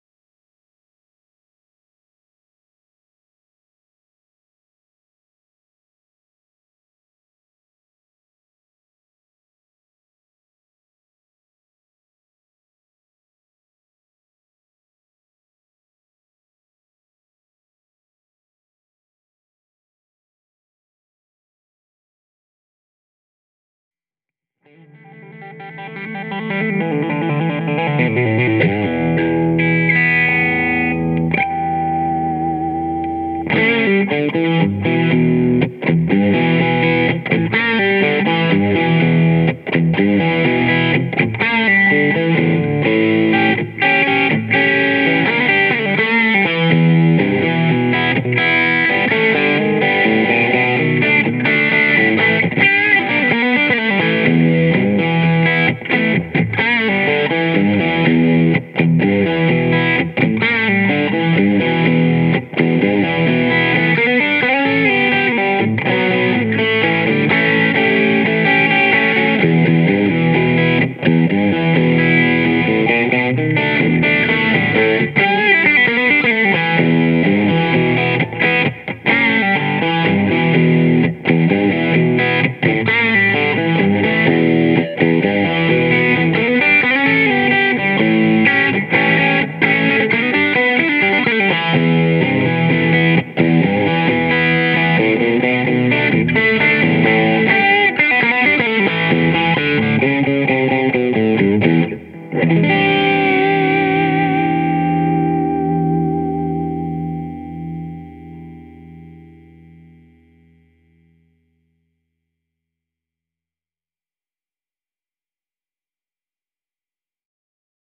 Me & my "Quad Cortex"...
Here's a short & sweet clip using the "Neural DSP Quad Cortex", and a Fender Bassman "silver-face" Amp capture  I downloaded from the Neural-DSP Cloud and adjusted to taste. The only thing added to this sound is a "UA Precision Limiter" to bring up the volume a bit.